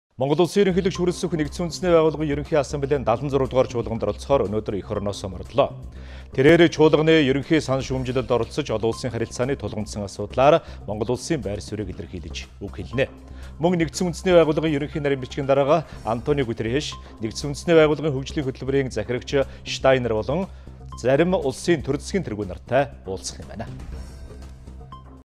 Монгол Улсын Ерөнхийлөгч У.Хүрэлсүх НҮБ-ын Ерөнхий Ассамблейн 76 дугаар чуулганд оролцоно
Монгол Улсын Ерөнхийлөгч У.Хүрэлсүх НҮБ-ын Ерөнхий Ассамблейн 76 дугаар чуулганд оролцохоор өнөөдөр /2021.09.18/ эх орноосоо мордлоо. Тэрээр чуулганы Ерөнхий санал шүүмжлэлд оролцож, олон улсын харилцааны тулгамдсан асуудлаар Монгол Улсын байр суурийг илэрхийлж үг хэлэх ба НҮБ-ын Ерөнхий нарийн бичгийн дарга А.Гутерреш, НҮБ-ын Хөгжлийн хөтөлбөрийн захирагч А.Штайнер болон зарим улсын төр, засгийн тэргүүнтэй хоёр талын уулзалт хийнэ.